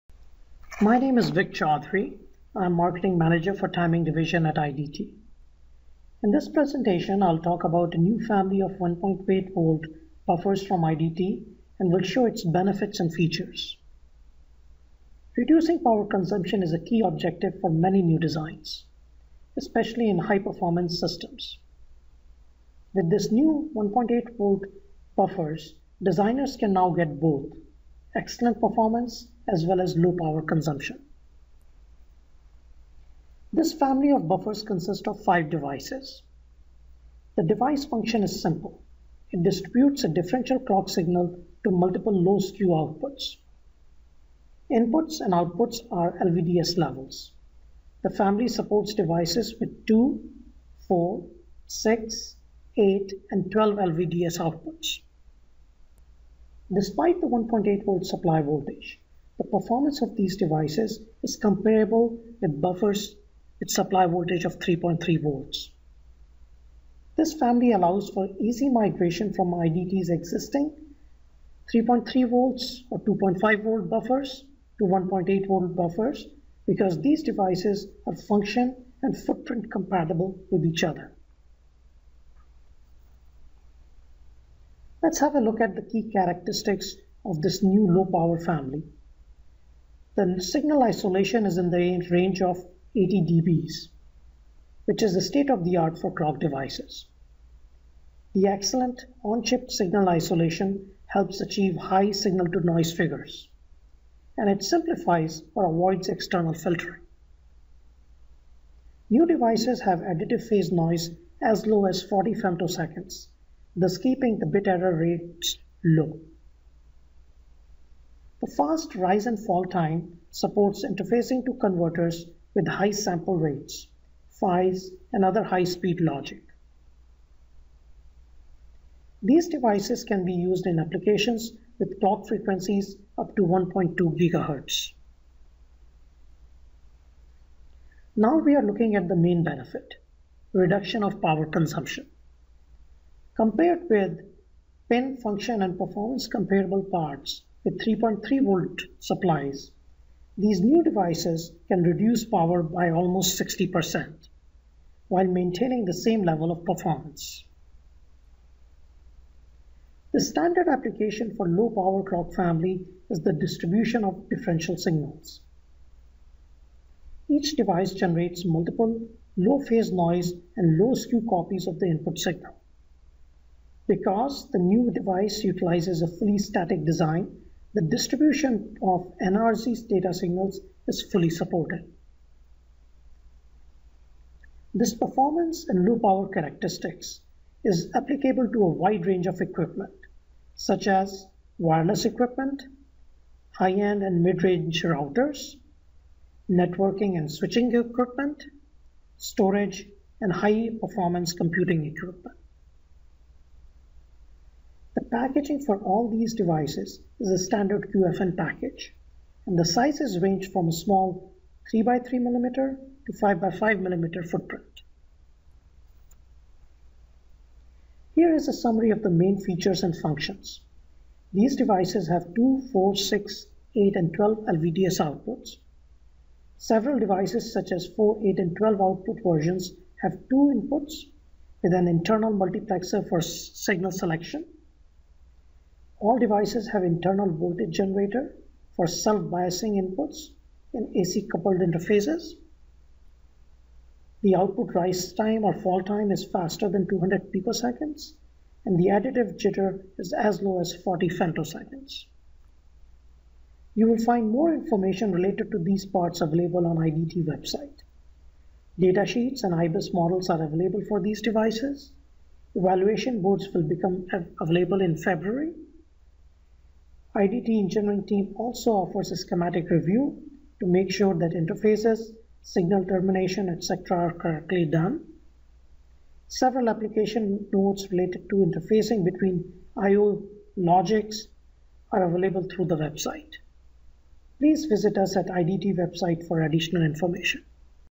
1.8V LVDS Clock Buffers by IDT: Low-power, High-performance
This video presentation will introduce users to IDT's 1.8 V LVDS clock fanout buffer family offering high-performance AC characteristics similar to that of 3.3 V devices. The new low-voltage fanout buffers enable customers to save up to 60% power and heat dissipation without sacrificing accuracy, bit error rates, or functionality.

Reducing power consumption is a key objective for many electronic designs, especially in high-performance systems. High-speed AC performance previously required the selection of high power consumption IC solutions. With IDT's 1.8V differential clock buffer family, designers can now get both: excellent AC performance and low power consumption.

The IDT 8P34S family devices are currently sampling to qualified customers and are available in 16- to 40-lead VFQFN packages. Customers may select from the IDT 8P34S1102i (2 output), 8P34S1204i (4 output), 8P34S1106i (6 output), 8P34S1208i (8 output) and 8P34S1212i (12 output) devices. Presented by Vik Chaudhry, Marketing Manager at IDT. For more information about IDT's industry-leading portfolio of fanout buffer products,